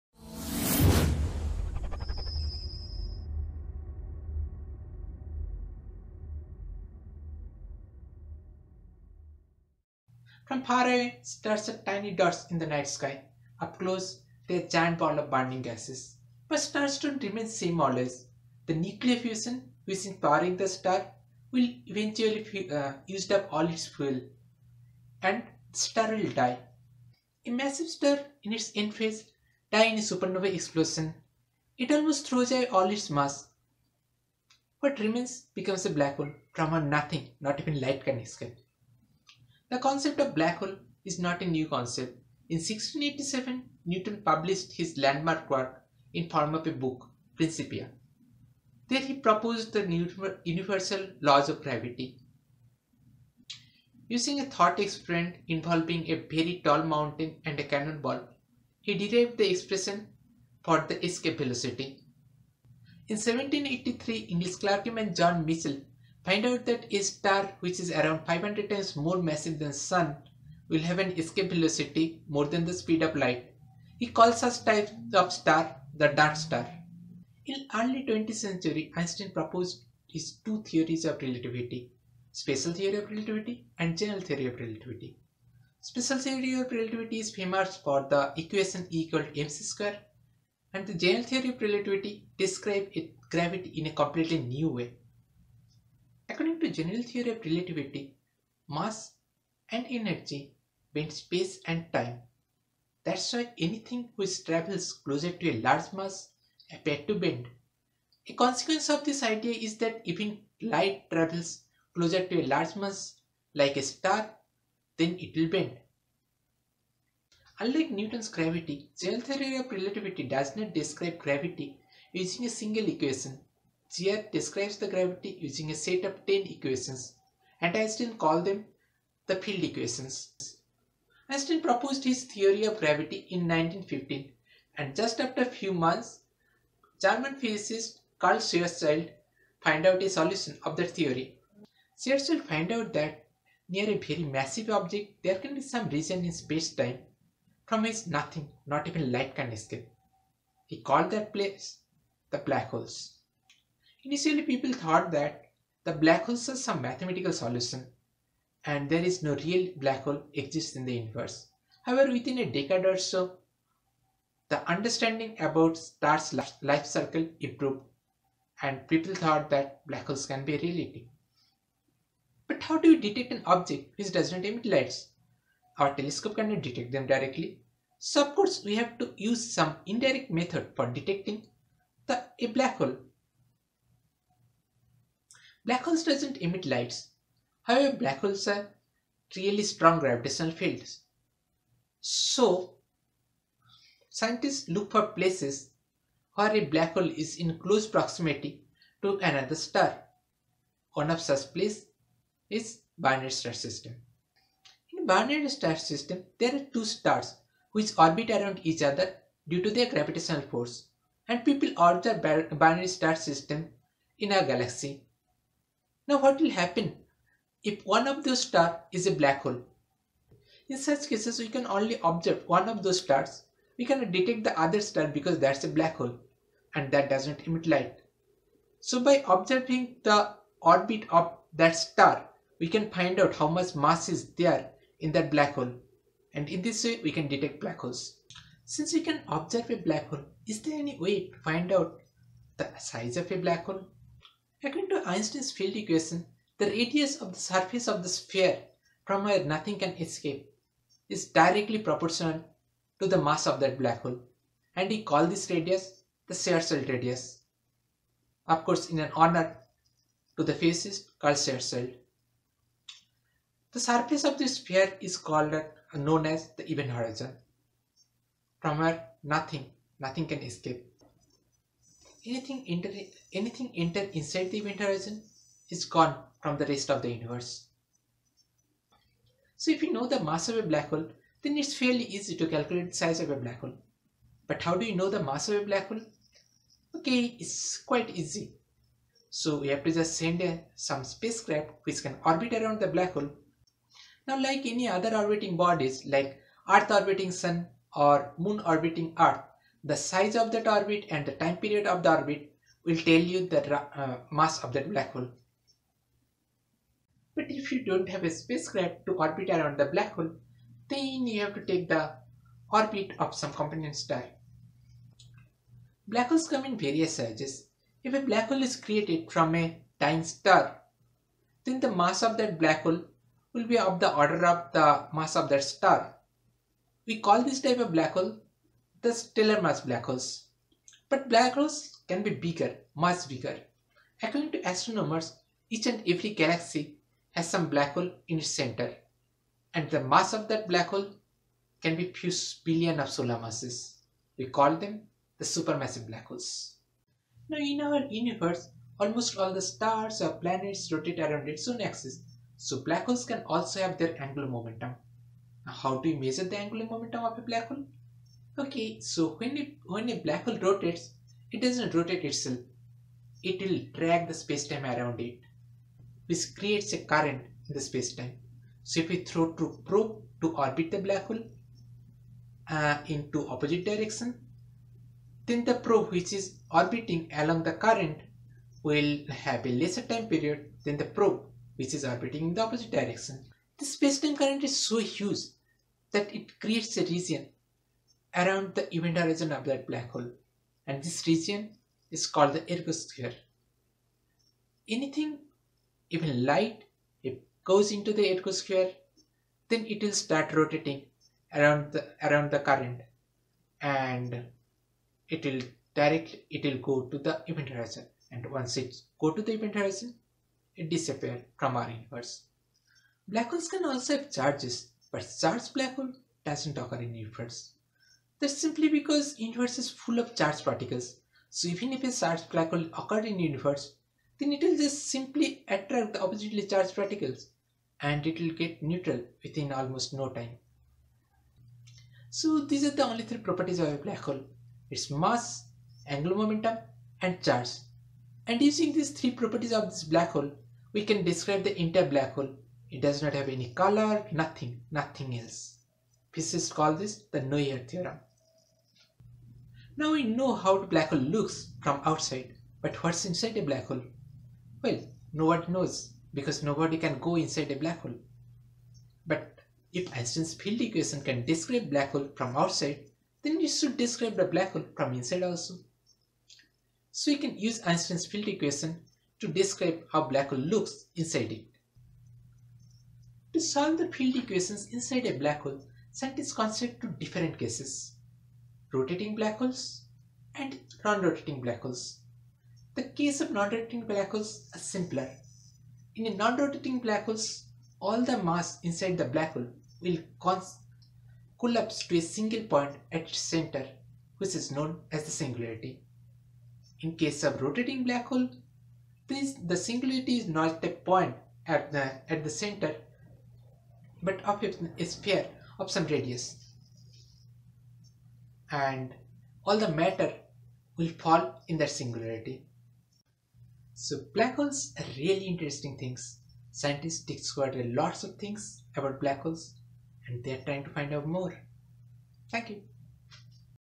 WTFScience #4 : What are Black Holes?
In WTF Science we describe different scientific concept in a simpler way. Today's topic is Black Hole..

by : Santanu Das